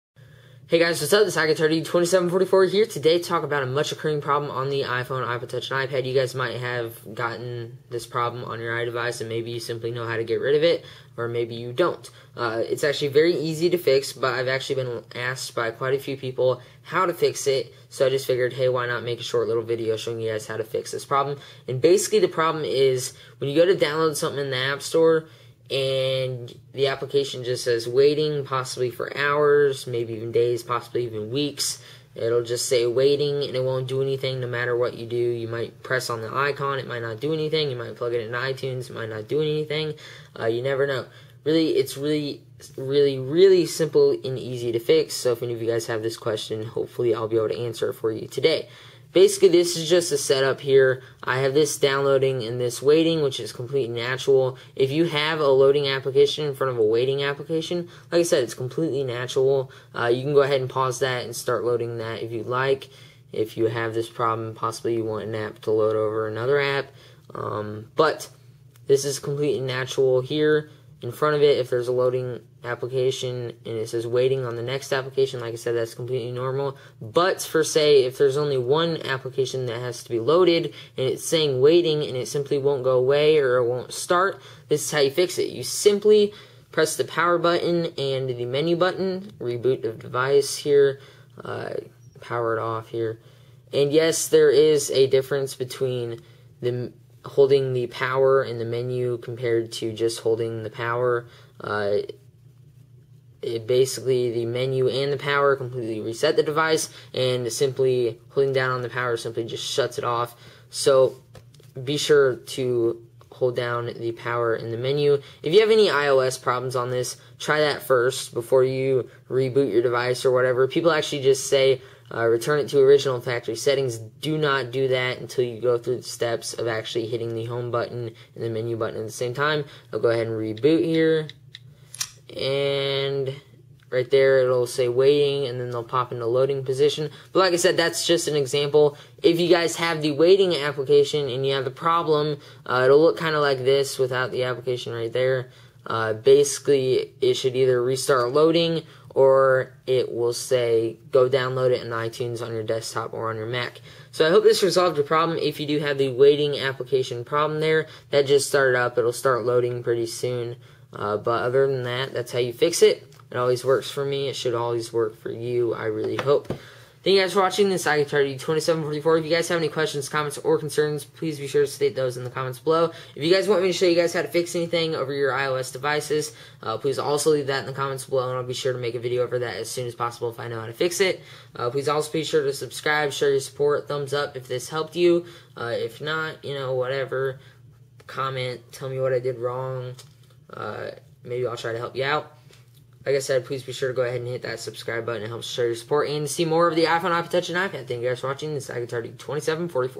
How to get rid of apps that wont load on iPhone, iPod Touch, and iPad
I have received questions regarding how to get rid of apps on your iDevices  in which are not loading or are always on waiting. This is a very simple and easy way to reset your app download and or get rid of the waiting application. Its simple yet, people don't know how to do it. Personally, I didn't know how to do it until I played around with it for a while. Now, I want to share the simple way to fix this with you all. I hope this has hellped you. If there are any questions, comments, or concerns please feel free to state though's below.

fix waiting apps on iphone